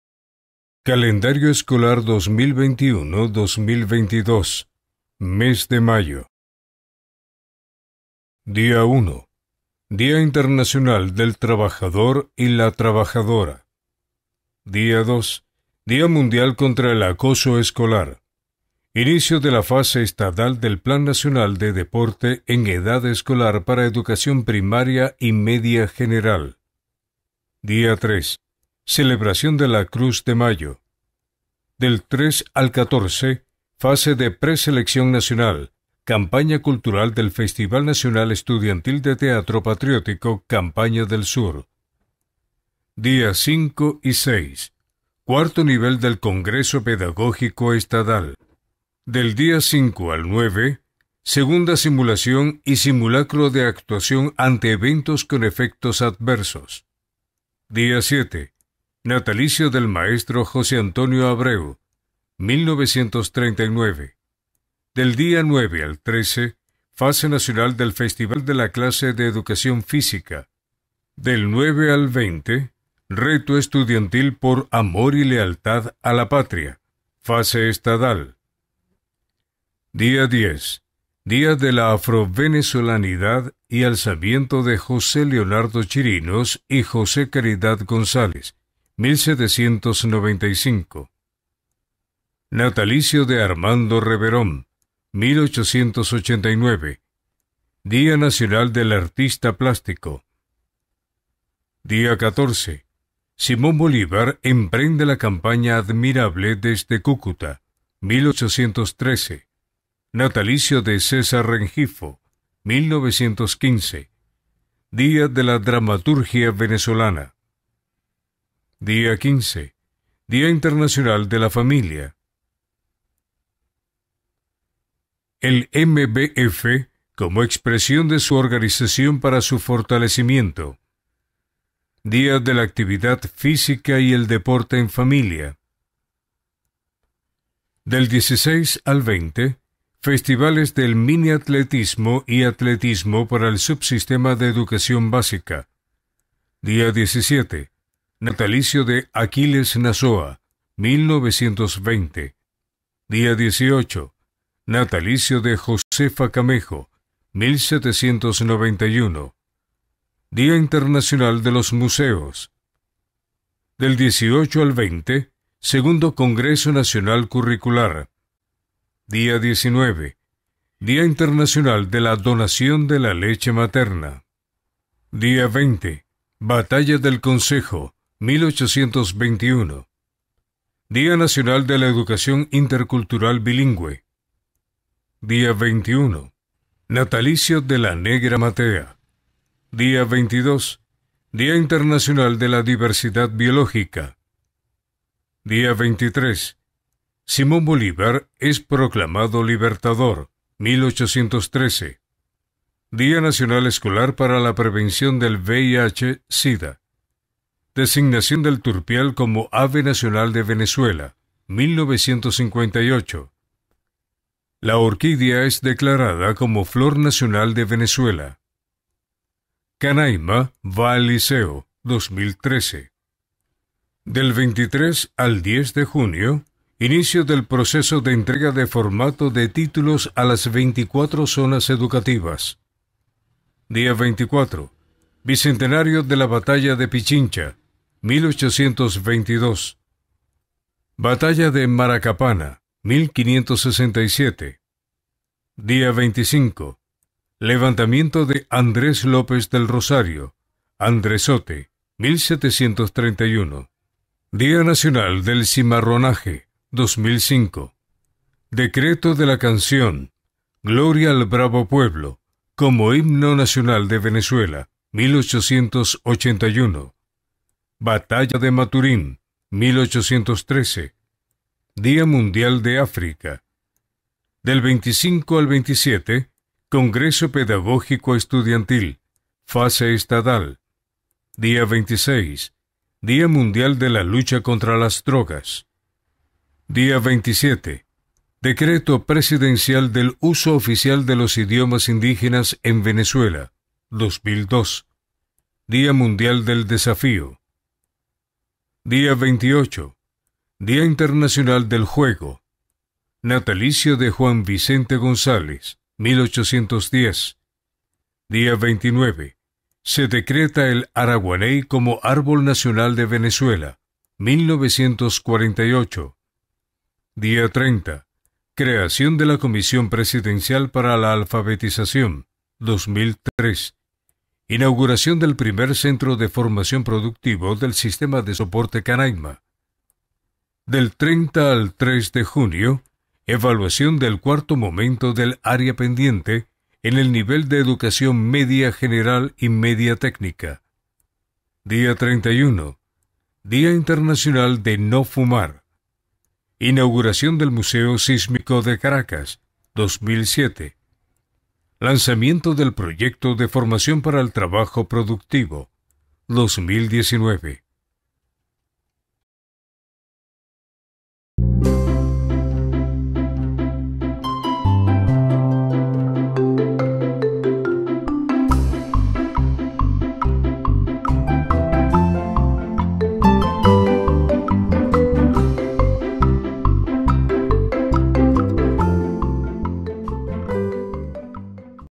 CALENDARIO ESCOLAR - MAYO 2022 / Ministerio del Poder Popular para la Educación (MPPE)
El Ministerio del Poder Popular para la Educación de la República Bolivariana Venezuela, a través de la Dirección General de Recursos para el Aprendizaje, publica el Calendario escolar oficial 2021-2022